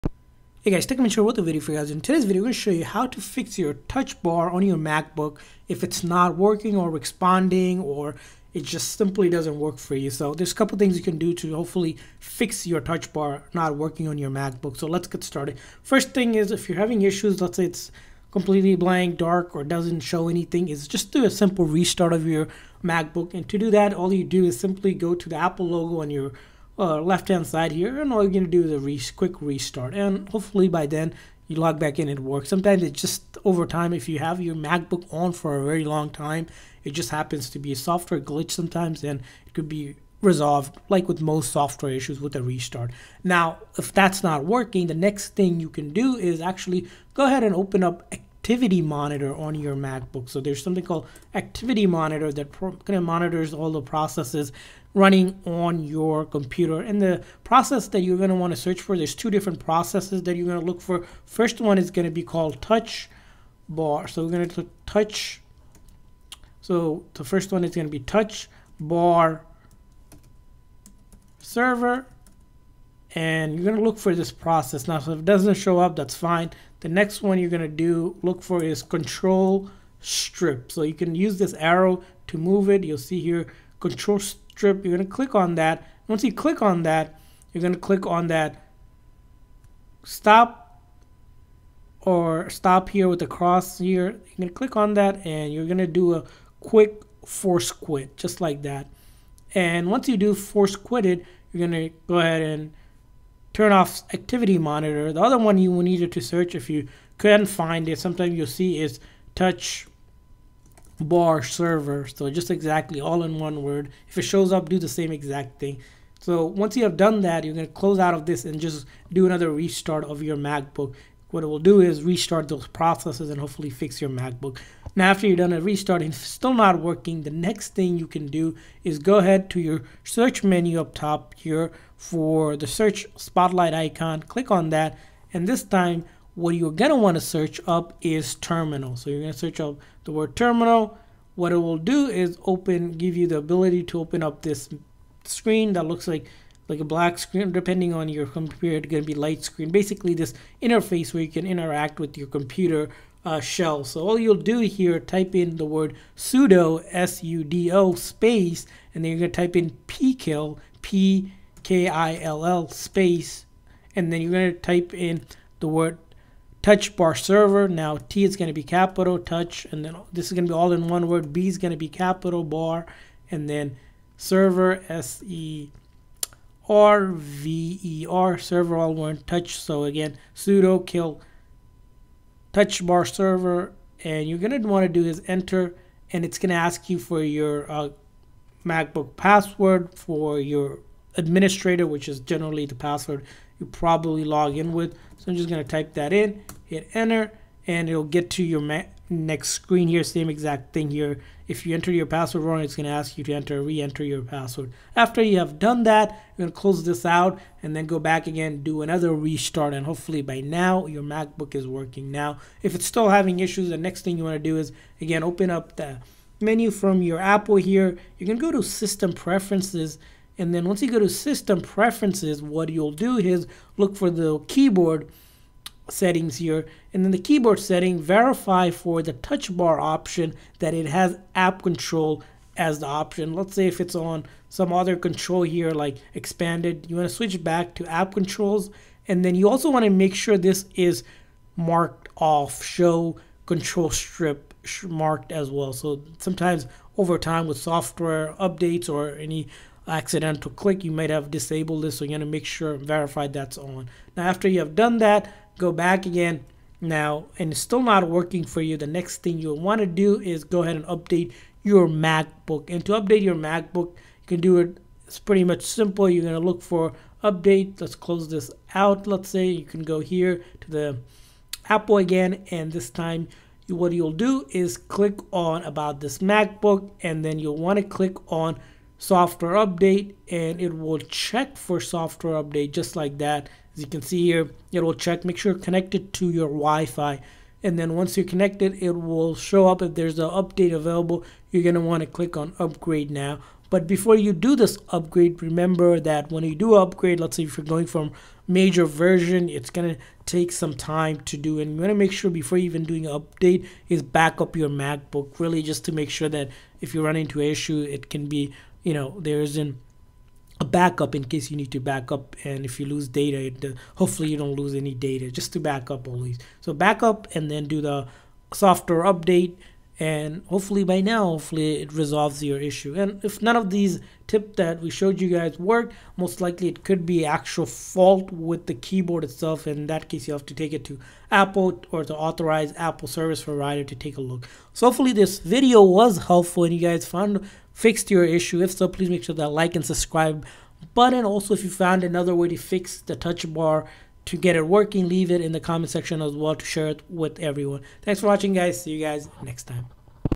How to Fix MacBook Touch Bar Not Working
In this video we show you how to fix your MacBook touch bar not working anymore. If the touch bar is black, blank, unresponsive or completely not working anymore, you can do the following steps to fix the MacBook Pro touch bar again.

Discounted New Macbook Pro 2020 (US)
Discounted New Macbook Pro 2020 (UK)
Discounted New Macbook Pro 2020 (Canada)
USB-C for MacBook